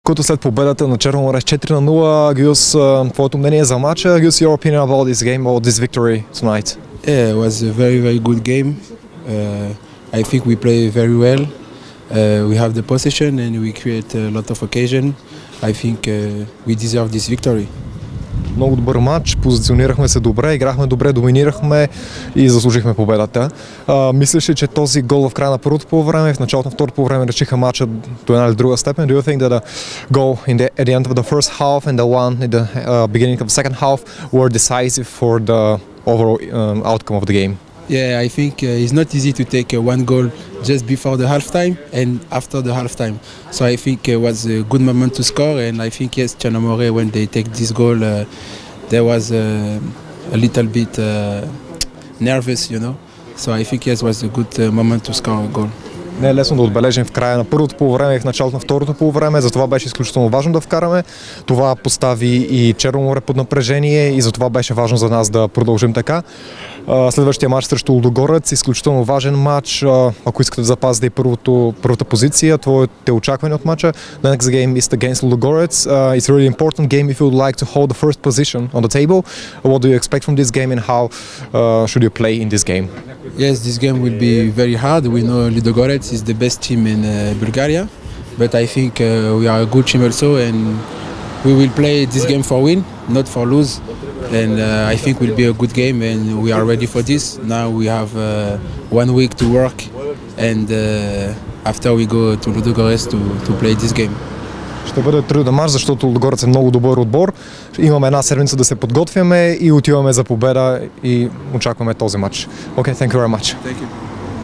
Гаюс Макoута след Берое - Черно море
Мнението на Гаюс Макoута след Берое - Черно море (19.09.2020)